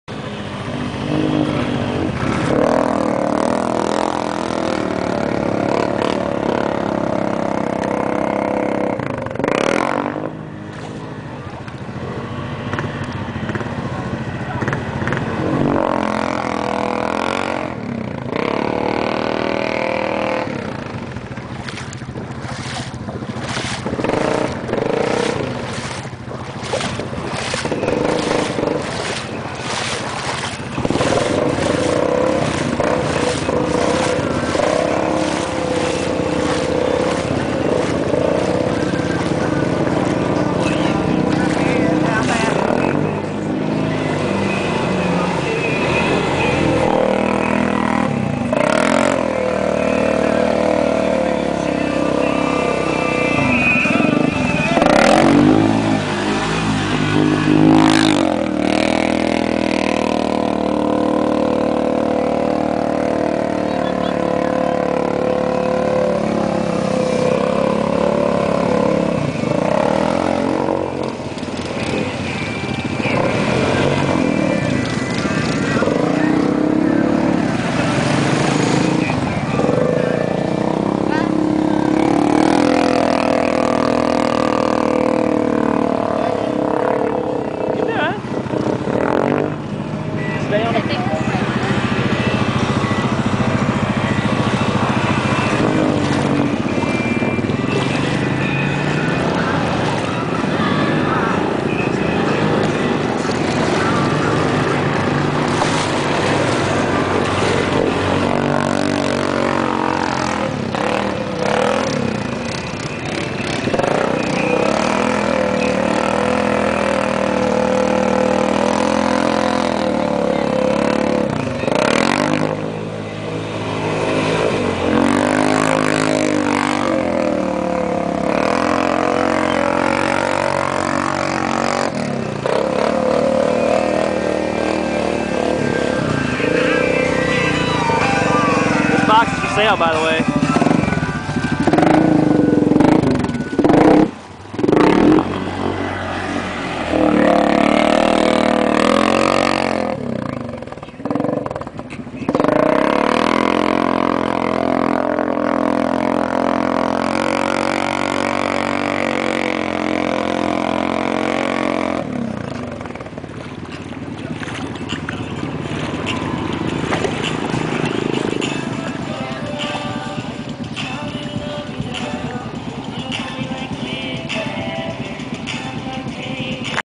naples water ride
another sunday down in naples with friends tried a few differnet water holes this time.